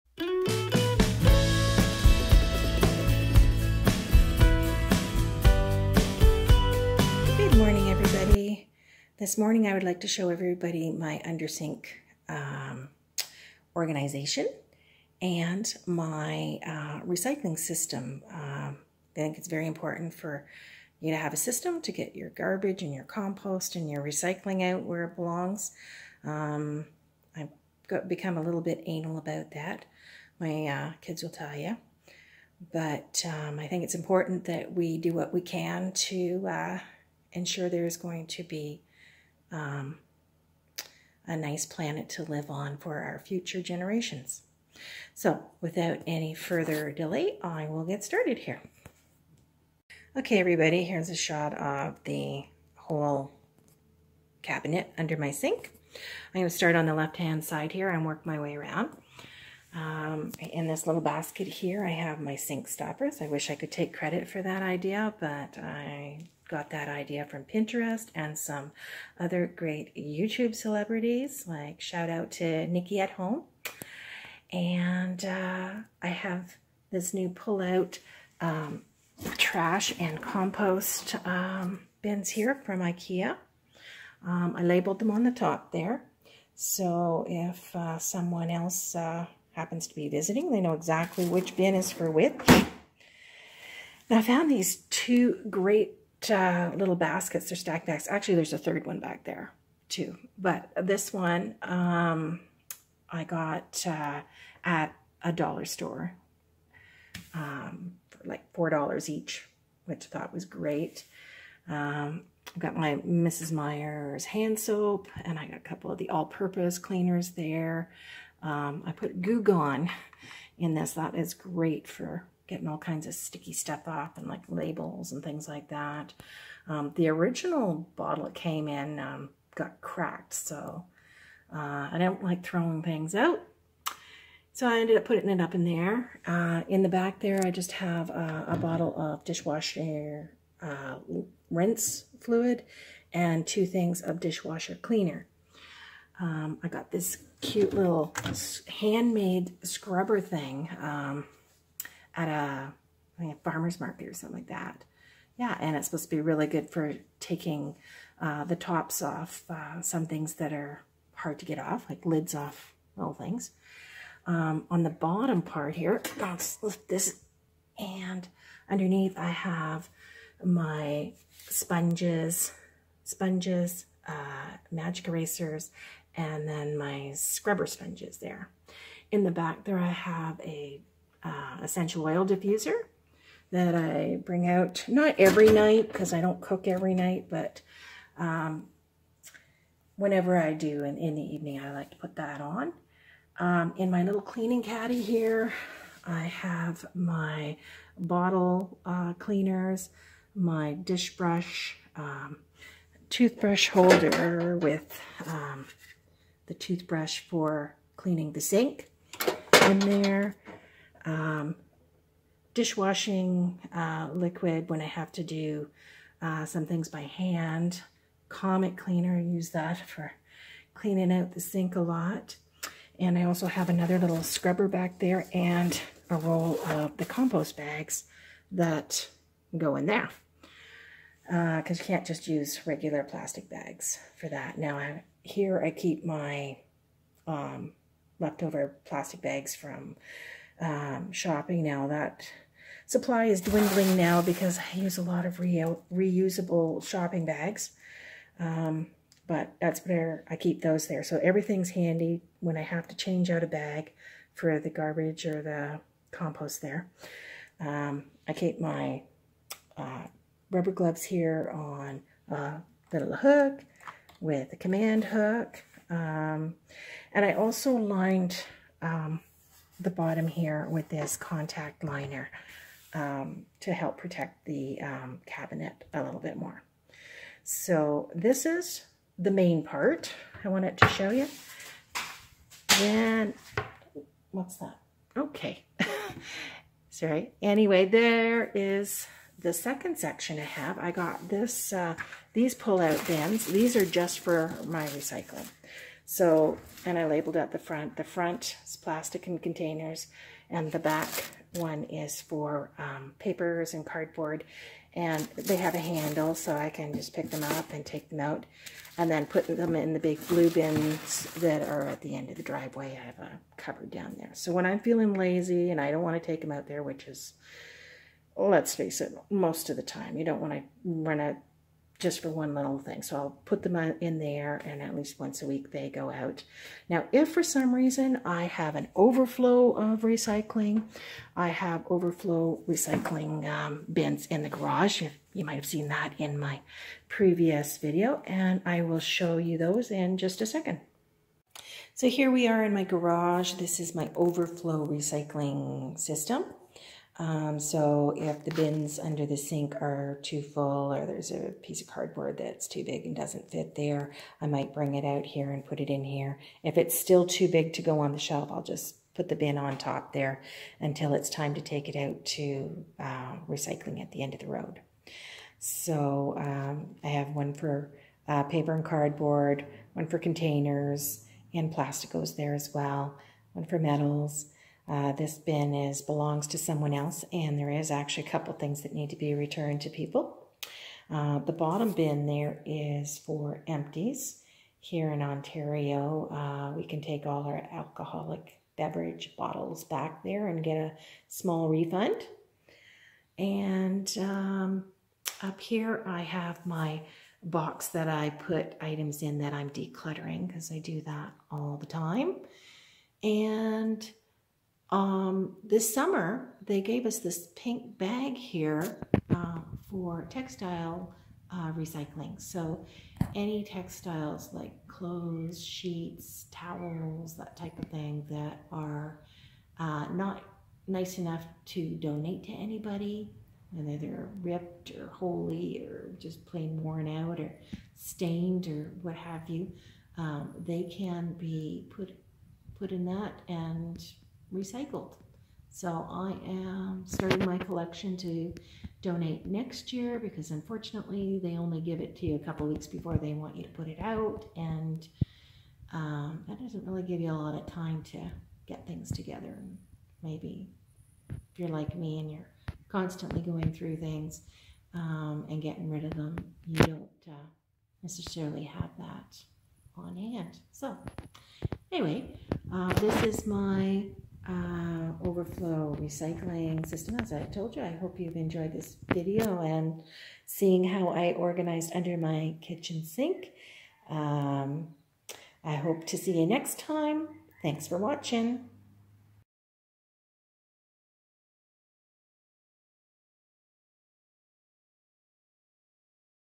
Under Kitchen Sink Organization & Waste & Recyling System
Organizing under the kitchen sink can be very challenging with all the pipes and drain,etc. Here I show how I organized everything and explain my system for organizing waste and recycling disposal. Where I live, now has recycling collection for almost everything, at least certain times of the year. Some of those things that only get collected periodically still need to have a place to keep until I get rid of them.